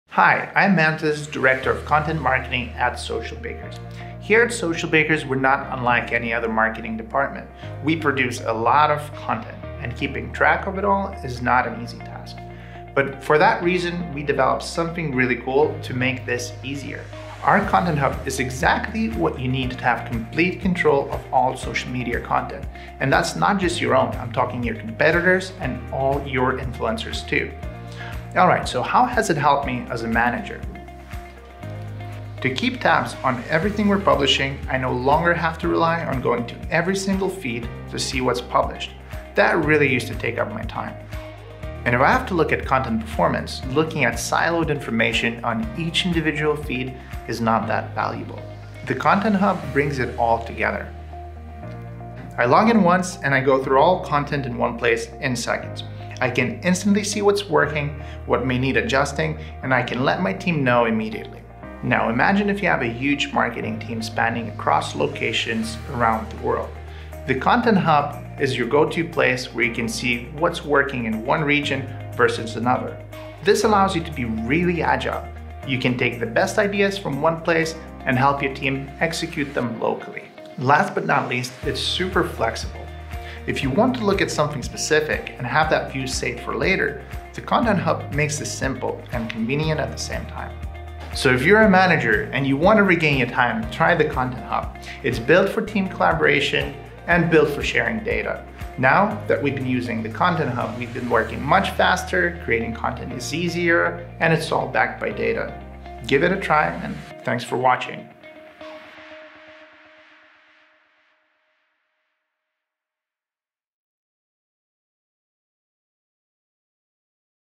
Content Hub for Content Marketing Directors
Use A.I driven Content Hub to get insights and analysis from across all digital channels to help teams collaborate, plan, and create engaging content your audiences will love.